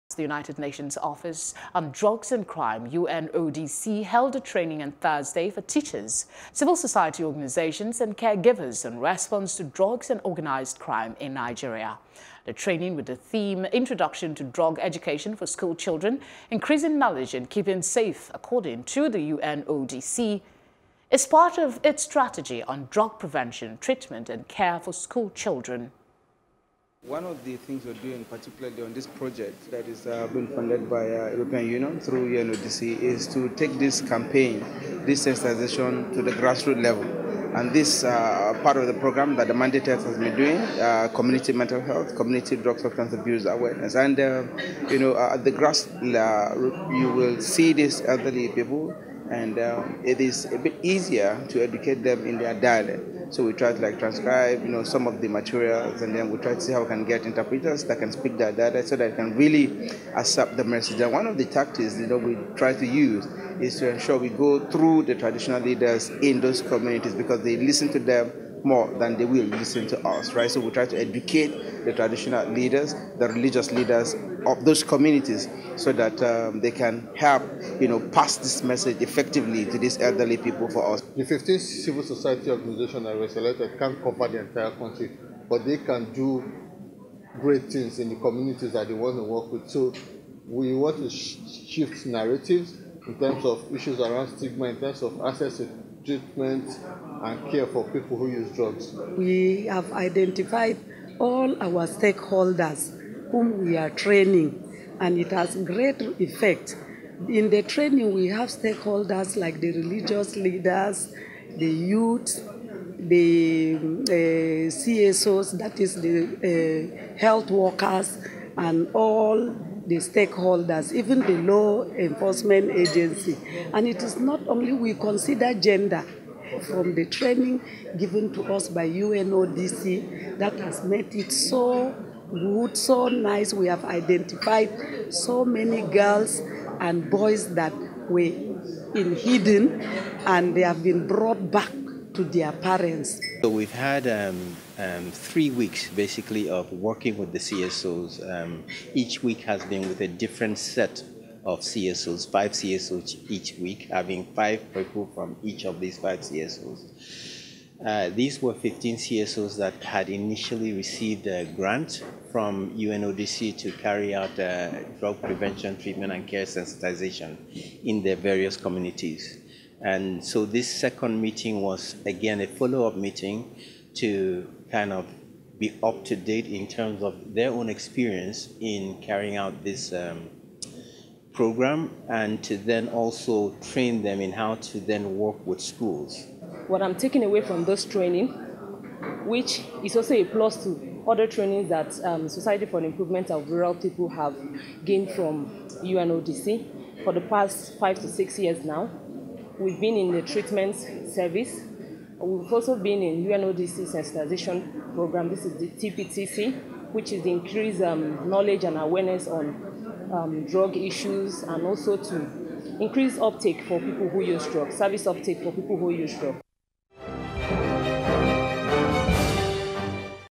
UNODC Advocates Drug Education For School Children
The United Nations office on Drugs and Crime UNODC held a training on Thursday for teachers, civil society organizations and care givers on response to drugs and organized crime in Nigeria.